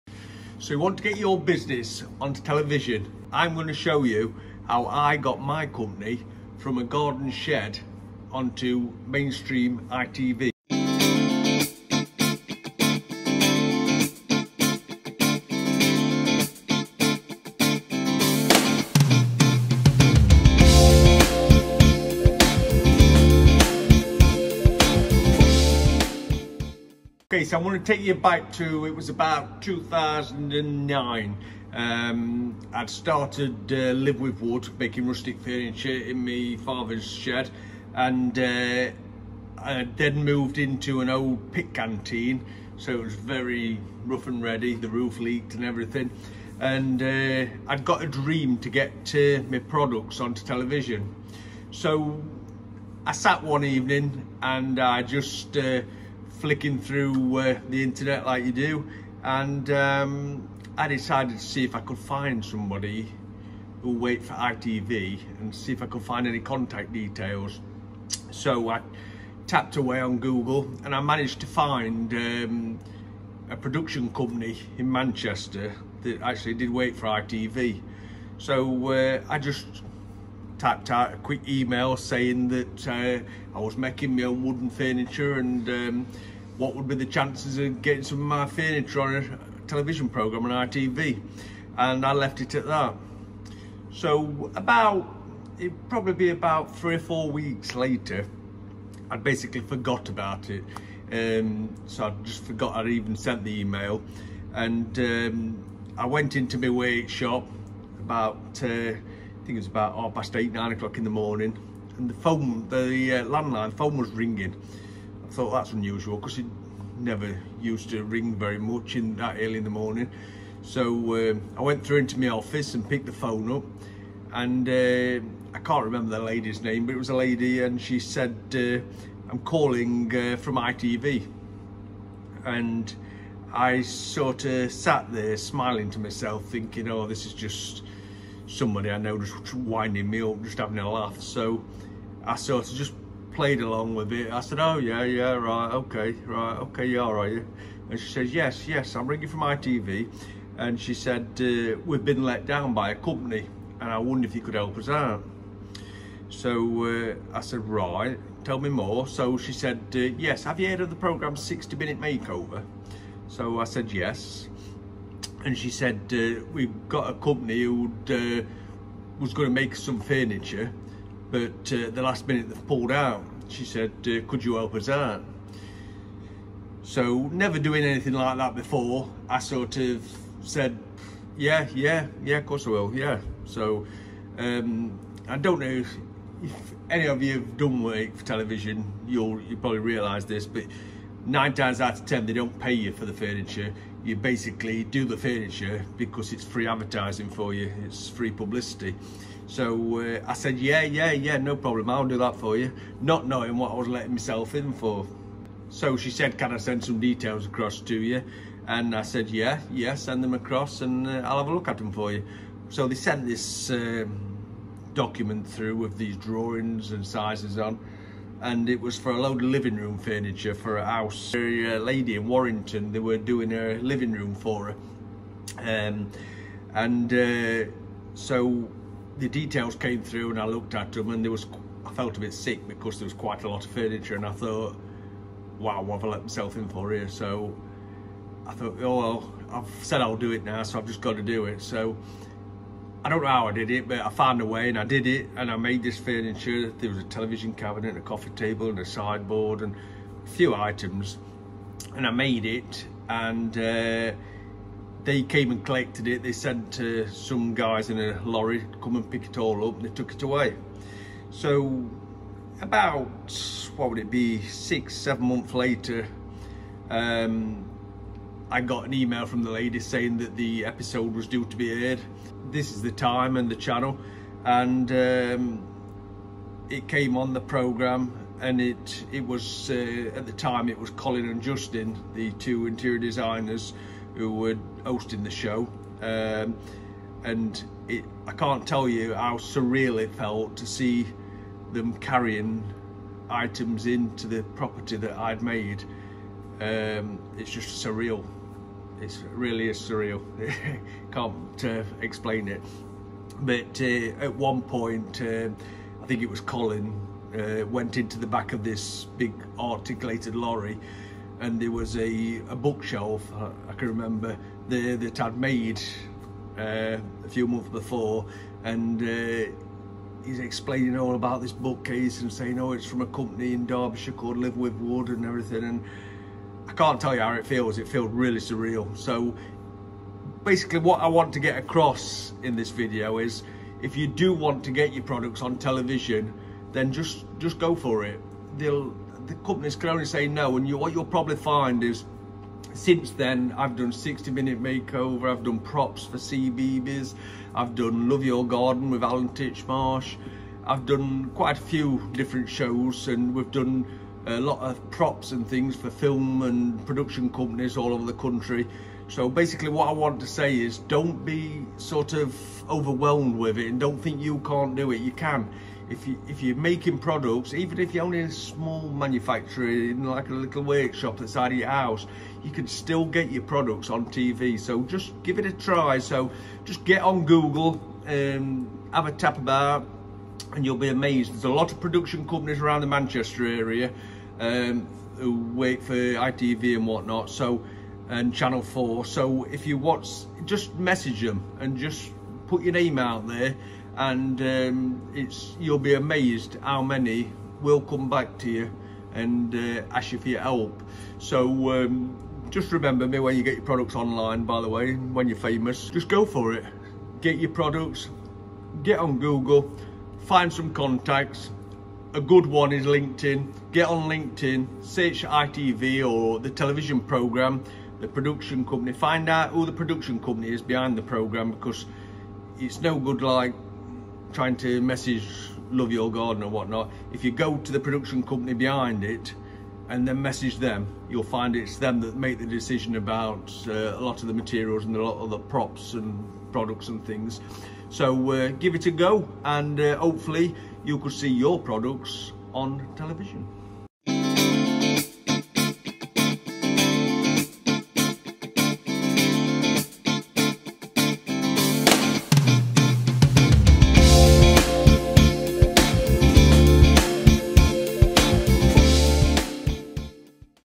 Get your WOODWORKING business on TV
I got my Woodworking business from a garden shed to NATIONAL TV.
In this video I am going to tell you how to do it..